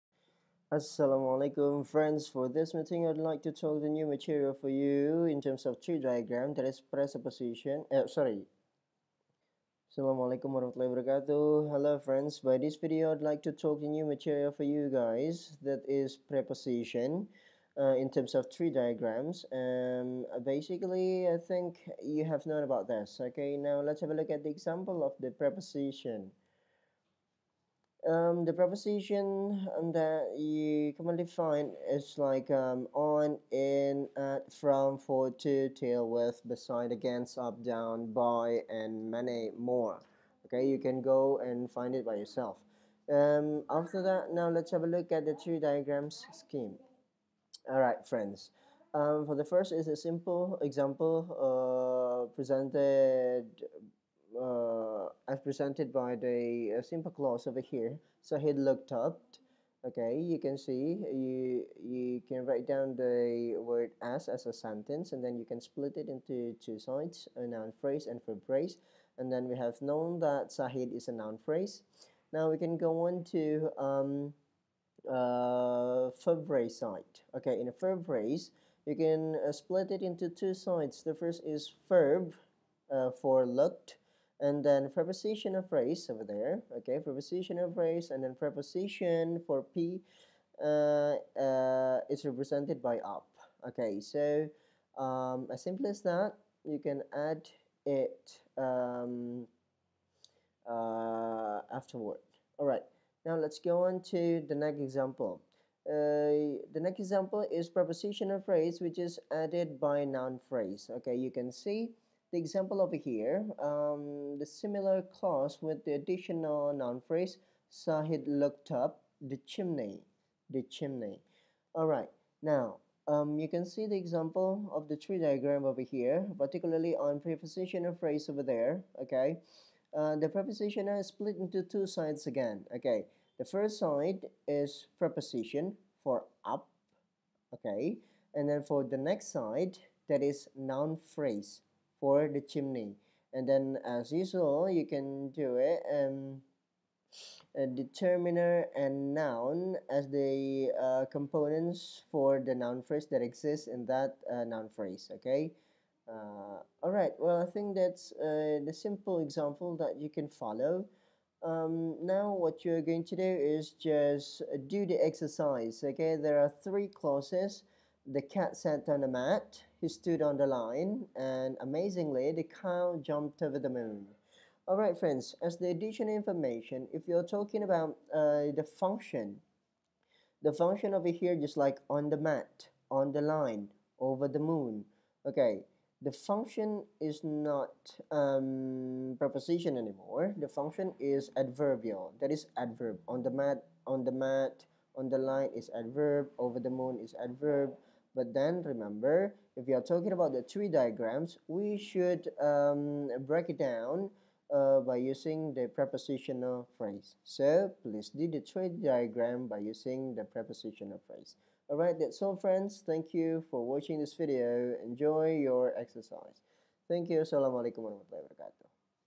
Preposition-Tree diagram
this video is one of materials for syntax subject. This preposition, in some cases, is functioned as adverbial such as in PP-P(+NP).